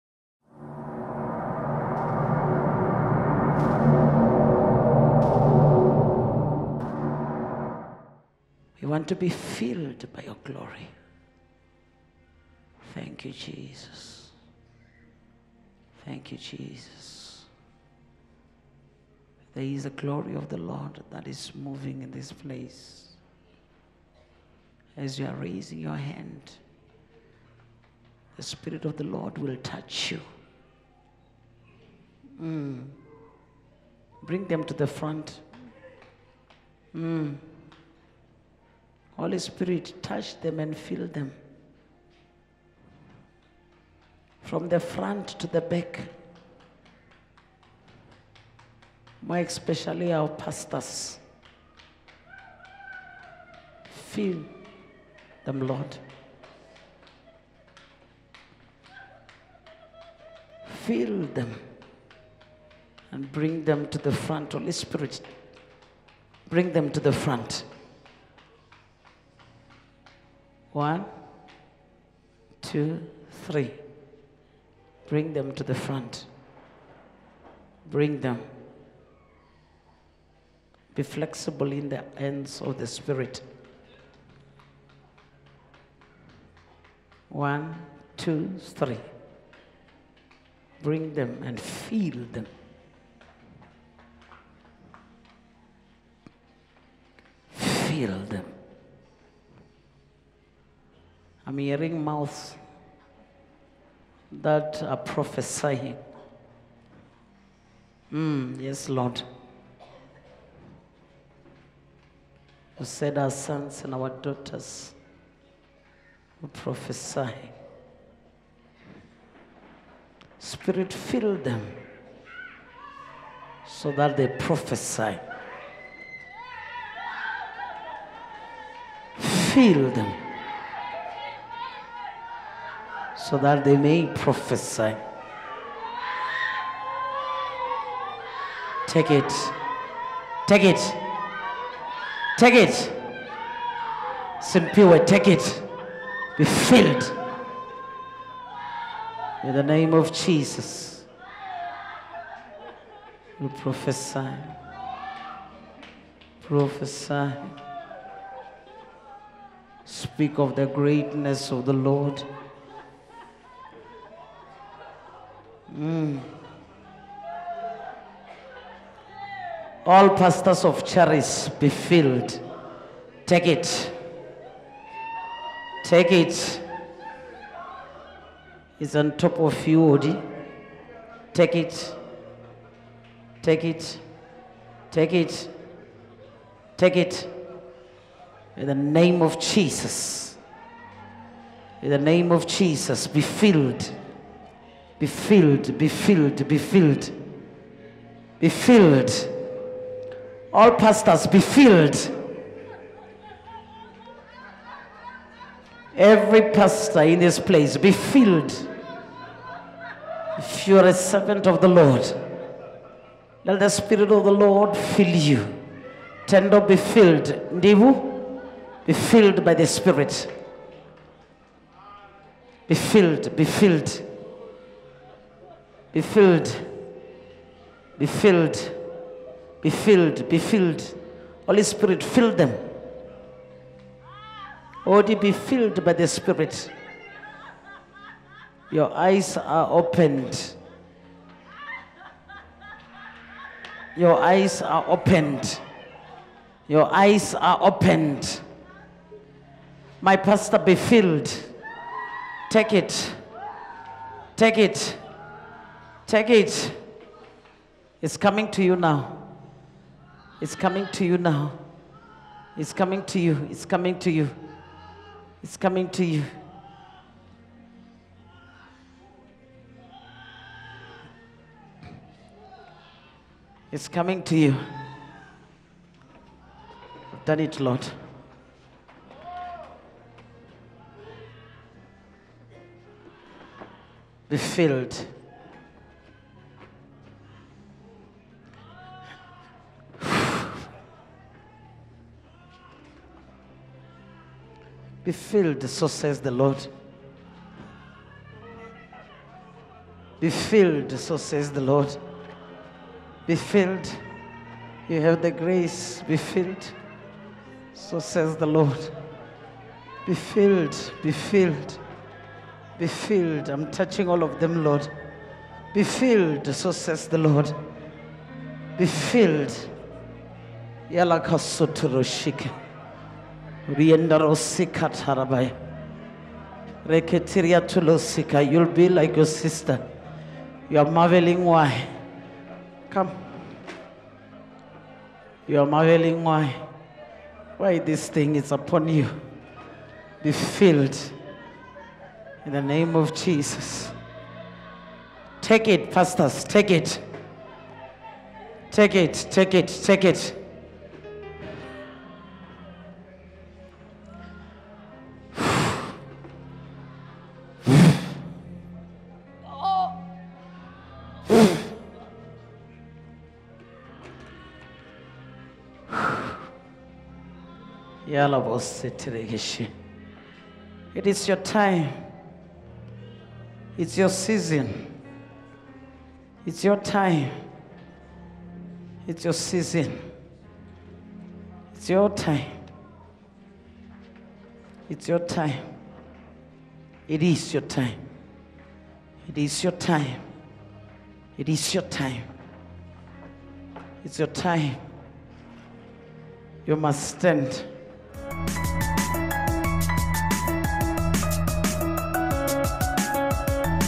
FILLED WITH THE GLORY TO PROPHESY
The Glory of the Lord descends in Charis Missionary Church, filling various individuals as well as Pastors for the distribution of the Prophetic Gift. Partake in this Grace as distance is not a barrier.